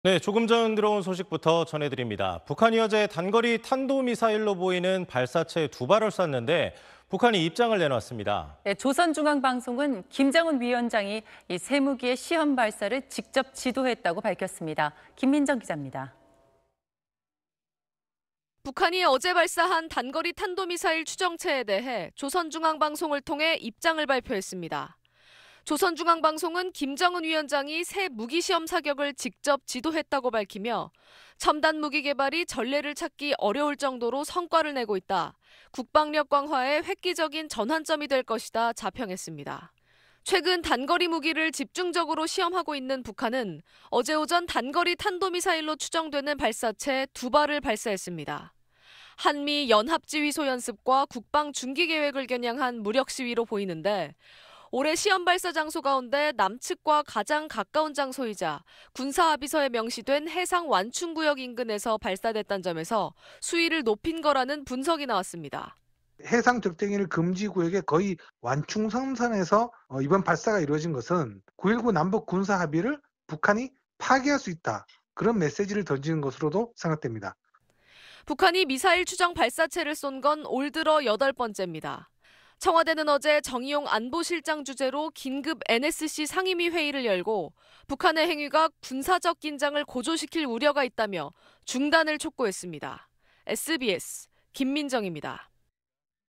북한 "김정은 지도로 새 무기 발사"…'남한 가까운 곳'서 수위 높여 / SBS
조금 전 들어온 소식부터 전해드립니다. 북한이 어제(16일) 단거리 탄도미사일로 보이는 발사체 2발을 쐈는데, 북한이 입장을 내놨습니다. 조선중앙방송은 김정은 위원장이 '새 무기'의 시험발사를 직접 지도했다고 밝혔습니다.
김민정 기자입니다.